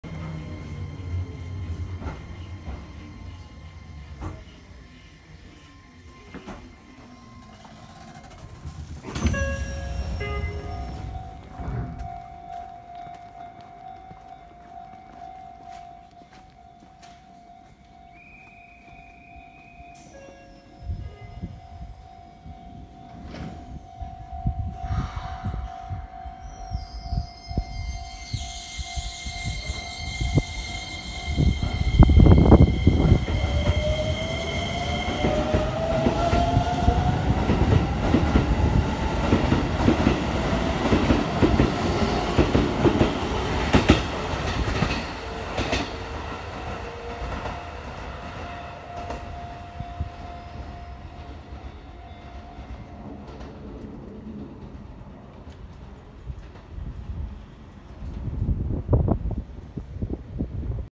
超小型LCDとはこれのこと 新京成N800形N848
2020年5月9日
昨日の動画にて登場当時で落胆したとコメントした液晶画面です。